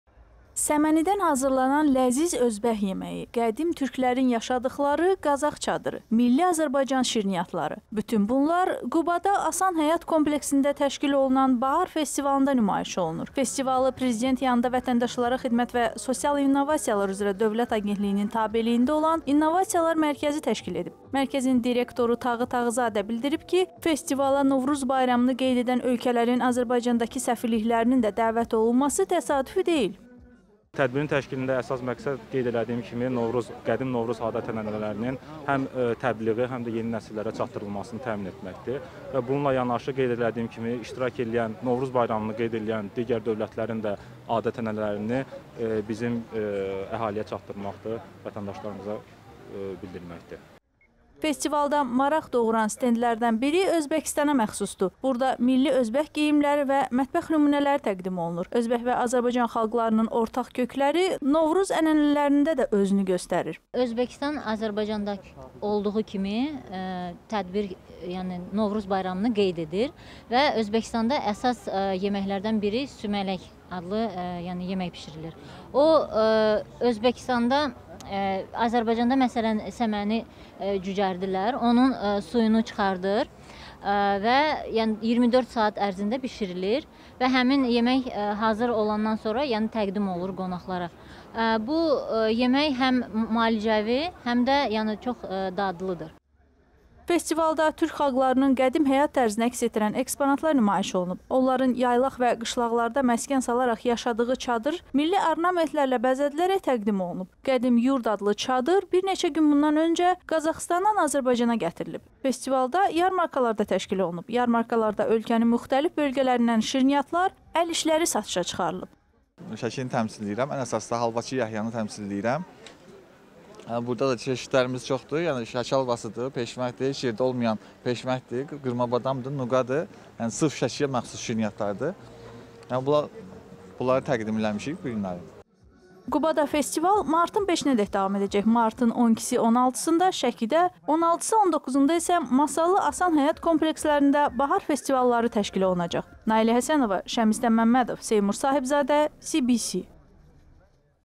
QUBADA BAHAR FESTİVALI KEÇİRİLİR
Səmənidən hazırlanan ləziz özbək yeməyi, qədim türklərin yaşadıqları qazax çadırı, milli Azərbaycan şirniyyatları… Bütün bunlar Qubada “ASAN Həyat” kompleksində təşkil olunan Bahar festivalında nümayiş olunur.  Festivalı Prezident yanında Vətəndaşlara Xidmət və Sosial İnnovasiyalar üzrə Dövlət Agentliyinin tabeliyində olan “İnnovasiyalar Mərkəzi” təşkil edib. Mərkəzin direktoru Tağı Tağızadə  bildirib ki, festivala Novruz bayramını qeyd edən ölkələrin Azərbaycandakı səfirliklərinin də dəvət olunması təsadüfi deyil.
Festivalda maraq doğuran stendlərdən biri Özbəkistana məxsusdur. Burada milli özbək geyimləri və mətbəx nümunələrİ təqdim olunur. Özbək və Azərbaycan xalqlarının ortaq kökləri Novruz ənənələrində də özünü göstərir.
Festivalda türk xalqlarının qədim həyat tərzini əks etdirən eksponatlar nümayiş olunub. Onların yaylaq və qışlaqlarda məskən salaraq yaşadığı çadır milli ornamentlərlə bəzədilərək təqdim olunub. “Qədim yurd” adlı çadır bir neçə gün bundan öncə Qazaxıstandan Azərbaycana gətirilib.
Festivalda yarmarkalar da təşkil olunub. Yarmarkalarda ölkənin müxtəlif bölgələrindən şirniyyatlar, əl işləri satışa çıxarılıb. Bahar festivalı martın 5-dək davam edəcək. Martın 12-16-da Şəkidə, 16-19-da isə Masallı ASAN həyat komplekslərində bahar festivalları təşkil olunacaq.